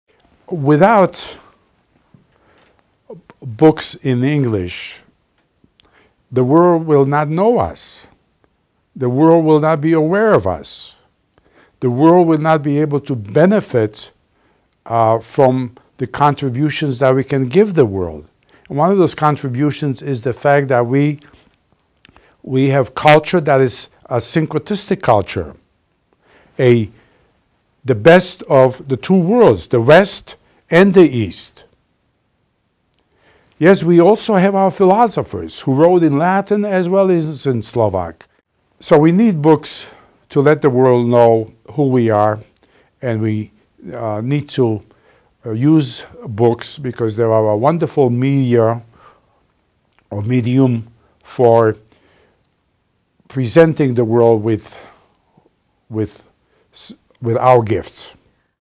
Ladislaus Bolchazy Clip 6
Ladislaus discusses the importance of books, especially English
translation, to Slovak and world culture